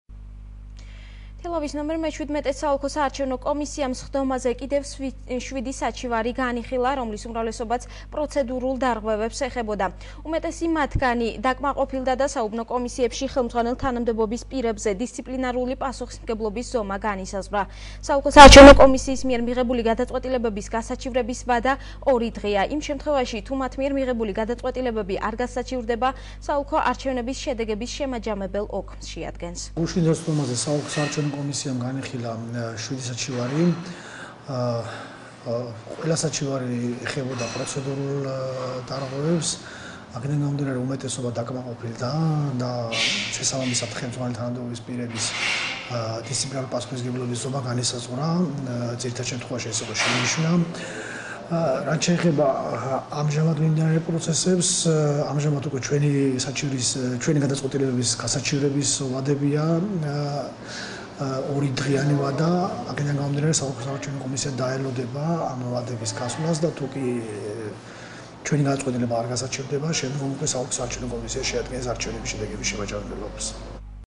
თელავის საოლქო კომისიაში საჩივრების განხილვა გრძელდება
თელავის მე-17 საოლქო-საარჩევნო კომისიამ სხდომაზე კიდევ 7 საჩივარი განიხილა, რომლის უმრავლესობაც პროცედურულ დარღვევებს ეხებოდა. უმეტესი მათგანი დაკმაყოფილდა და საუბნო კომისიებში ხელმძღვანელ თანამდებობის პირებზე დისციპლინარული პასუხისმგებლობის ზომა განისაზღვრა. საოლქო საარჩევნო კომისიის მიერ მიღებული გადაწყვეტილებების გასაჩივრების ვადა 2 დღეა. იმ შემთხვევაში, თუ მათ მიერ მიღებული გადაწყვეტილებები არ გასაჩივრდება, საოლქო არჩევნების შედეგების შემაჯამებელ ოქმს შეადგენს.